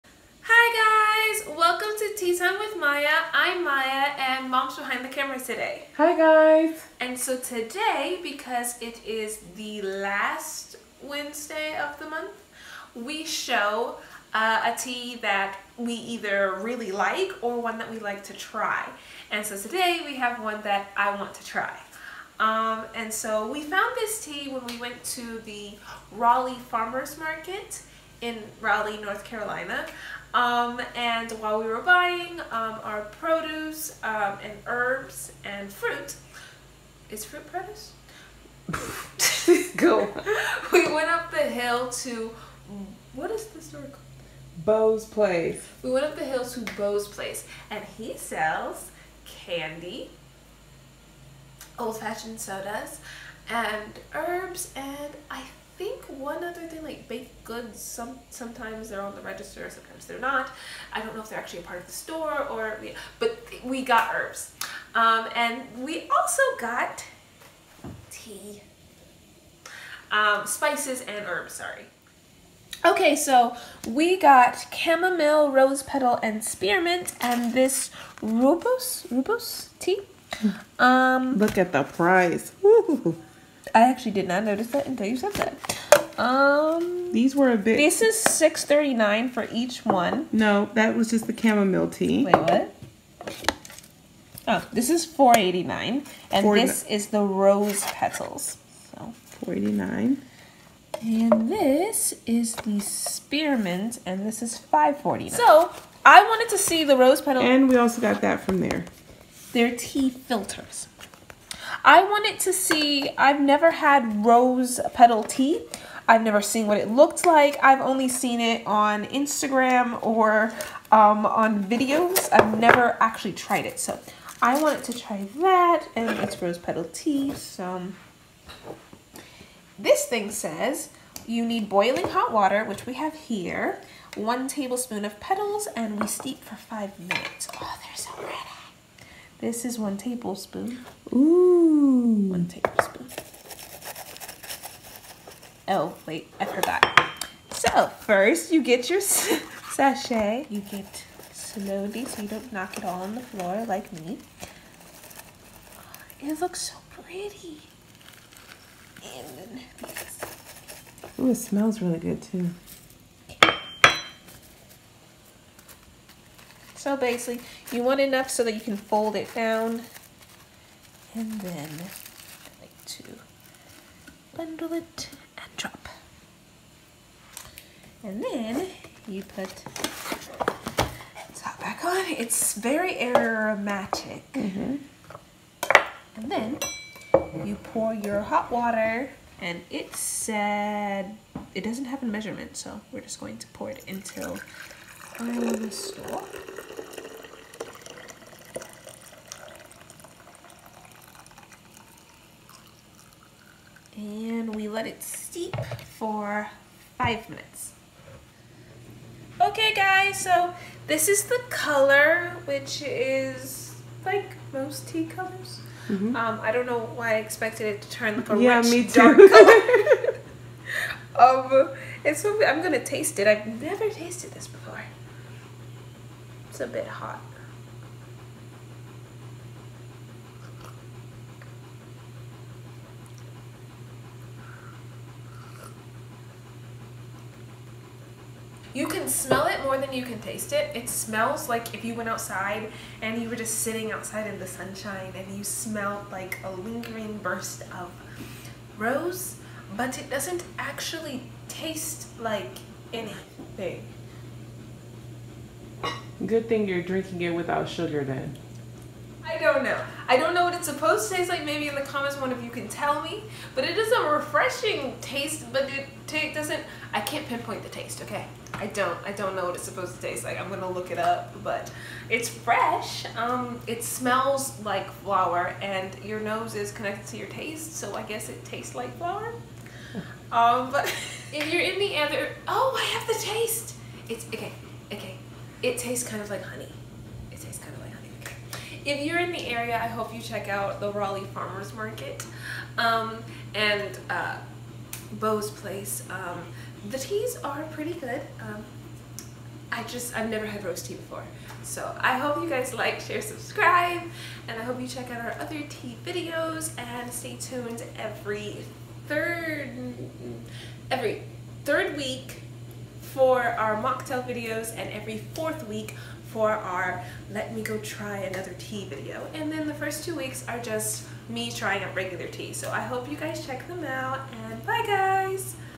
Tea Thyme With Maya| Beau's Place| Raleigh Farmers Market| Homemade Tea Blends| Spring| May 2022
Hello Everyone! We were looking the farmers market and found some different homemade brands of tea and we thought we would share them with all of you! The rose tea has a subtle  yet delicious flavor but the others have a robust aroma and taste.